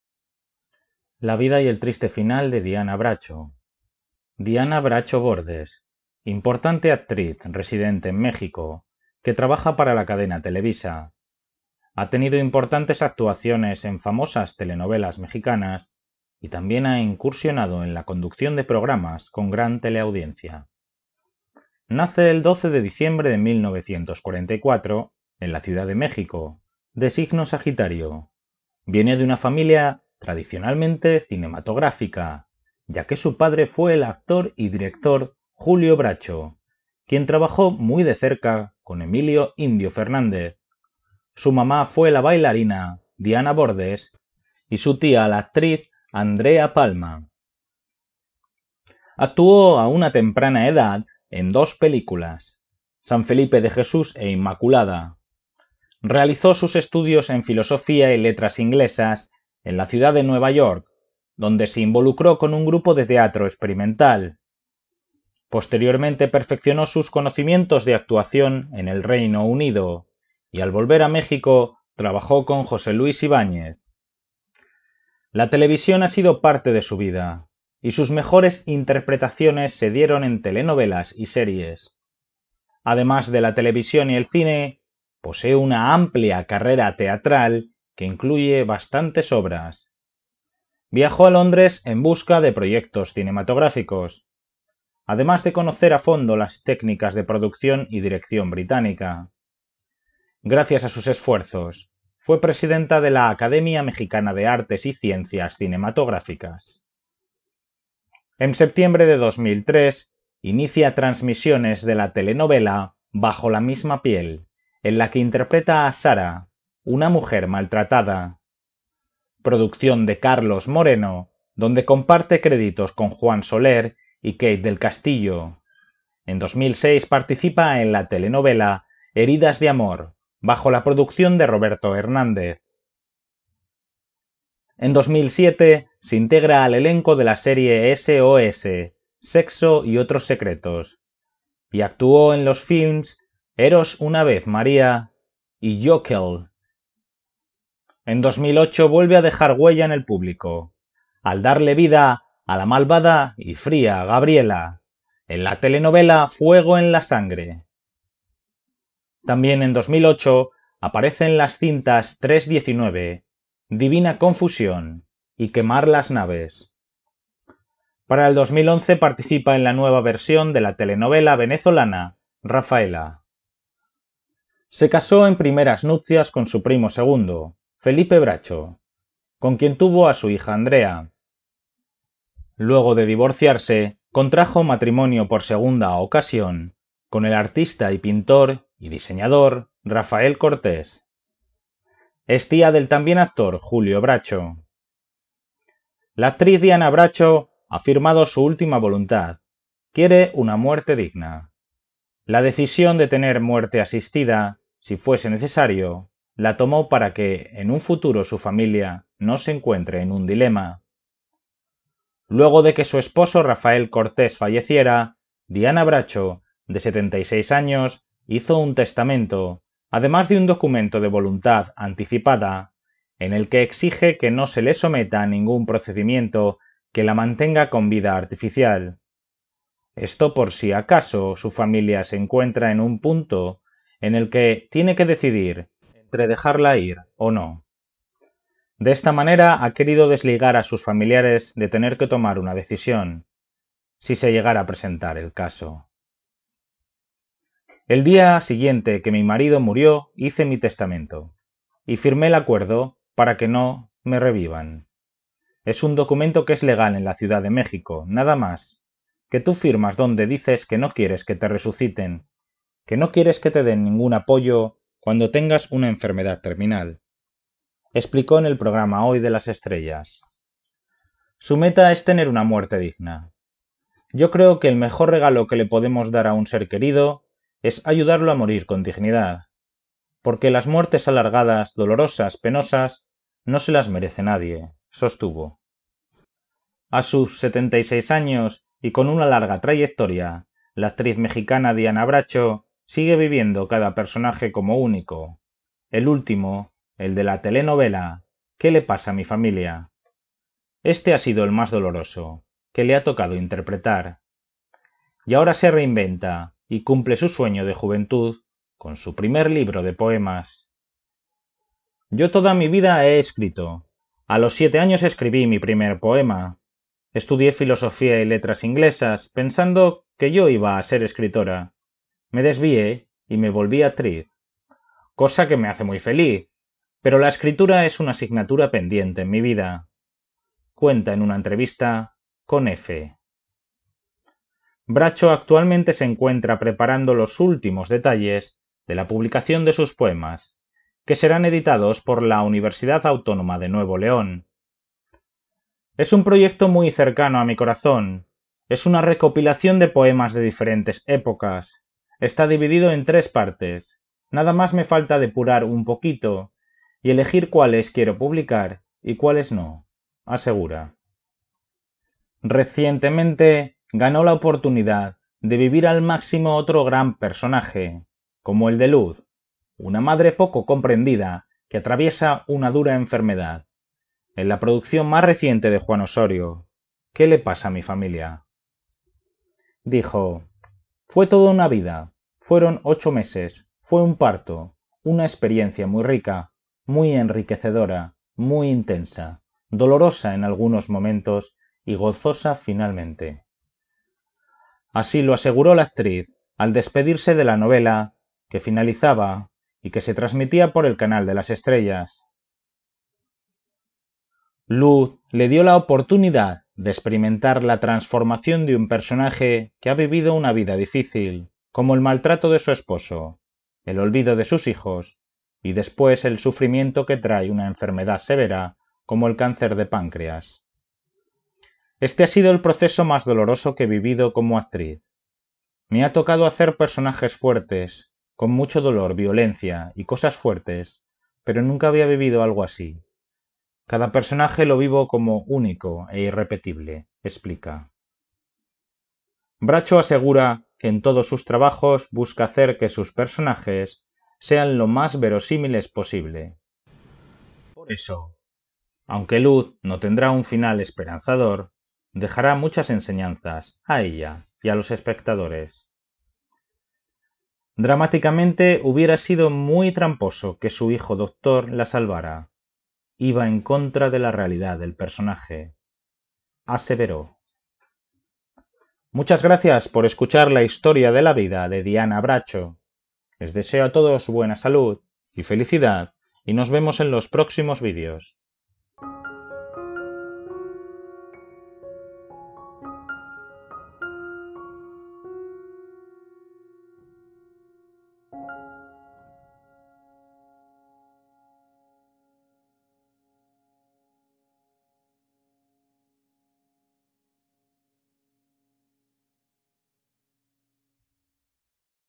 La Vida y El Triste Final de Diana Bracho
Mini biografía de la vida del Diana Bracho. Diana Bracho (Ciudad de México, 12 de diciembre de 1944) es una actriz mexicana de teatro, cine y televisión. Diana Bracho Bordes nació el 12 de diciembre de 1944 en la Ciudad de México. Proviene de una familia relacionada con el medio artístico.